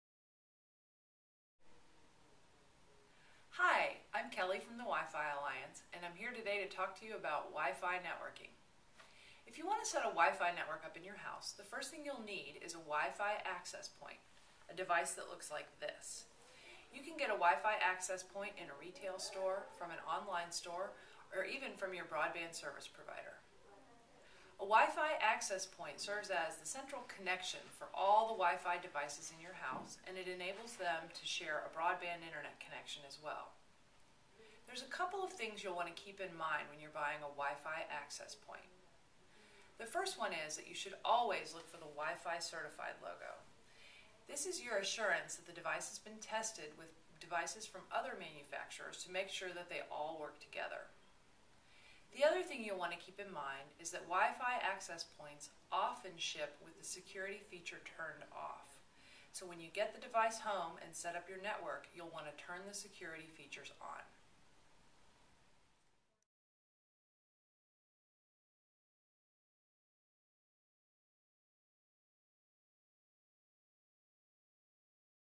Wi-Fi® Networking Basics
Kelly Davis-Felner, marketing director from the Wi-Fi Alliance® provides an overview of Wi-Fi networking.